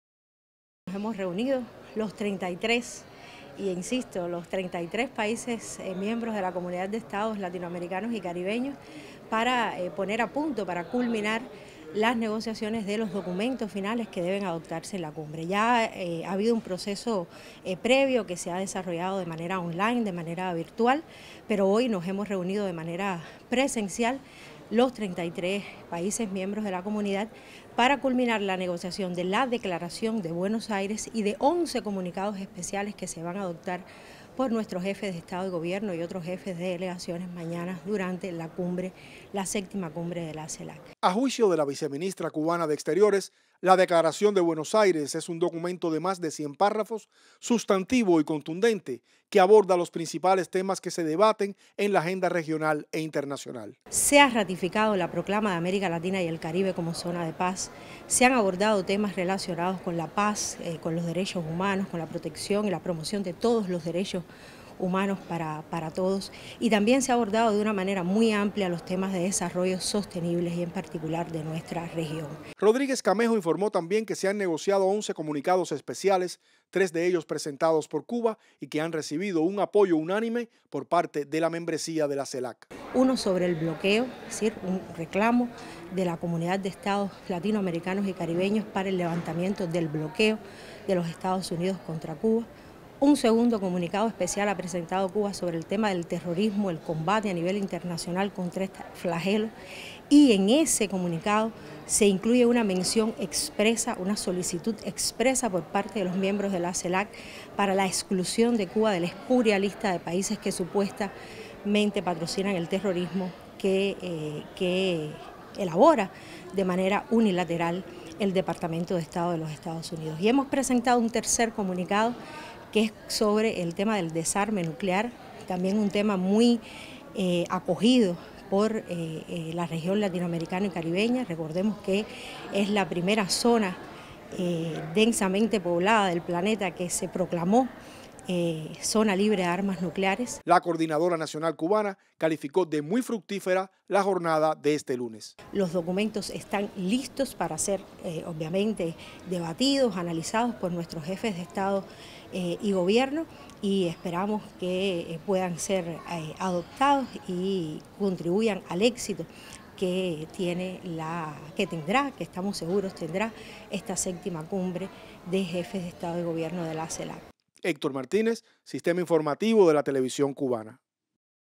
Todo listo para la VII Cumbre de la CELAC
Este lunes se pusieron a punto los documentos que se someterán a la consideración de las 33 naciones que conforman el mecanismo de integración.